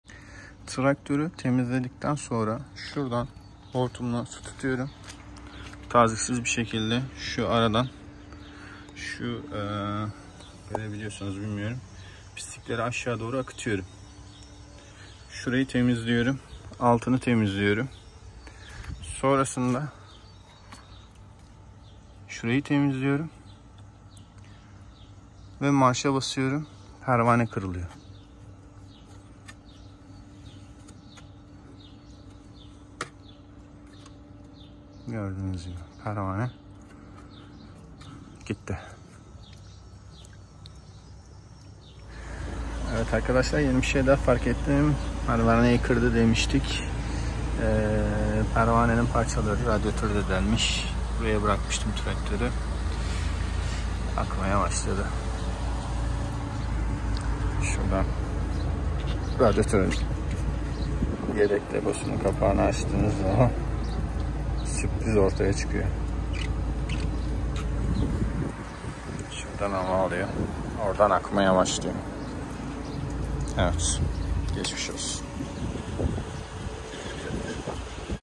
Newholland TD 100f pervane parçaladı radyatörü deldi. Pilastik pervane parçalandı.
SERVİS SADEÇE RADYATÖR KARŞILADI; içilik ve pervane davlumbaz antifriz benden !